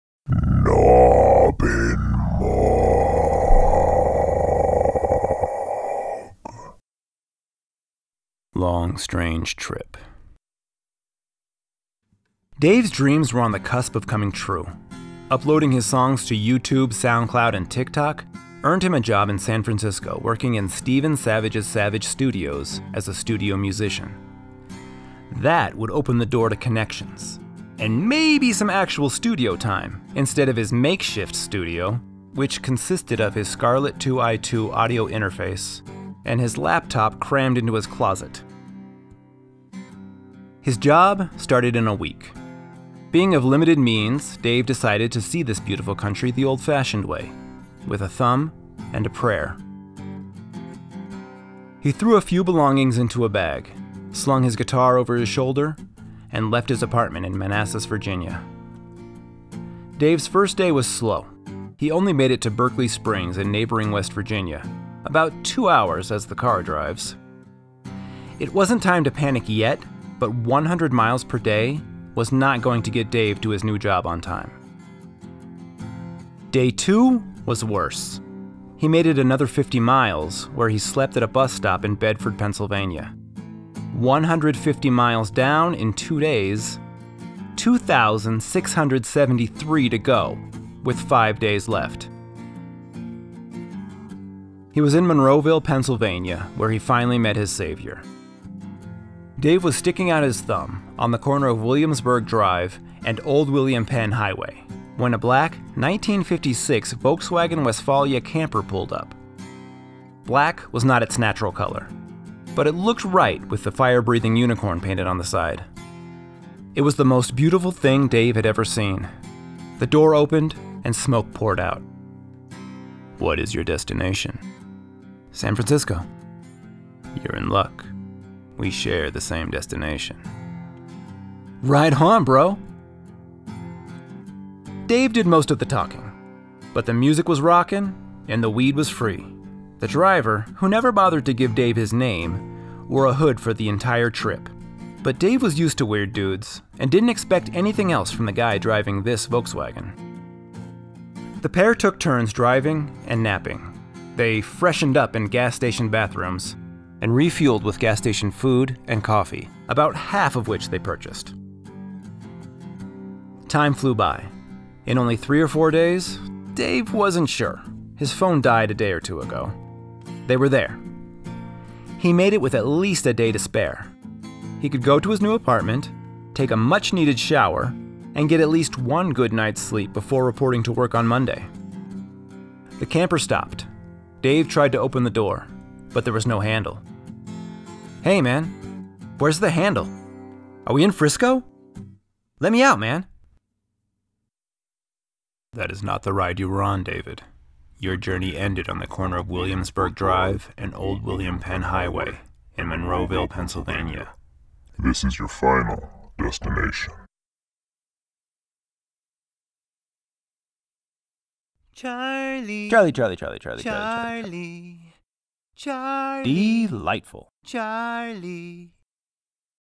Long Strange Trip
Dave's dreams were on the verge of coming true. He just had to find a way to get across the country. It turned out to be a long strange trip.

Written for The Story Vault curated by Chelsea Marie on Medium.